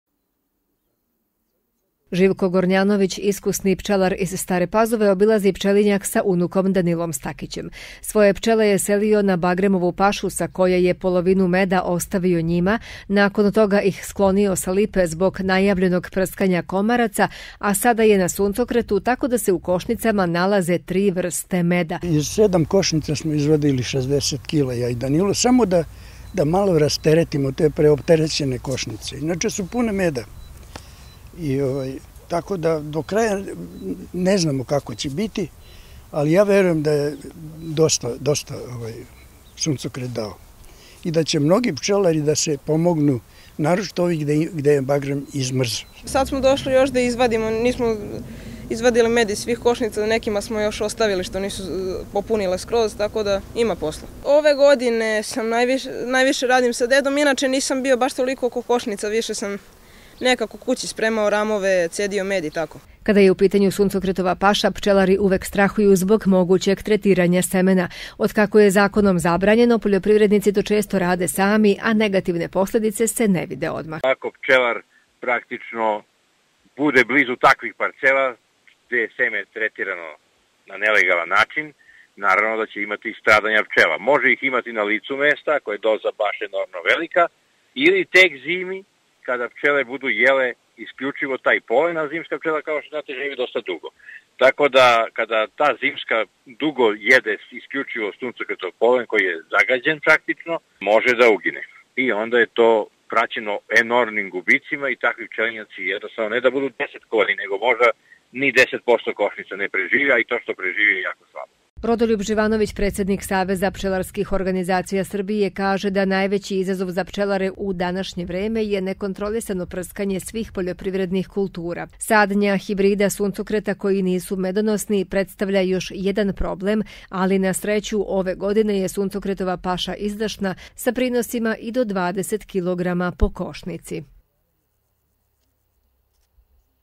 Suncokret odlično medi
Suncokret u Vojvodini ove godine izuzetno medi tako da su pčelari zadovoljni, ali strahuju od mogućeg trovanja pčela zbog neadekvatnog tretiranja seman ove uljarice.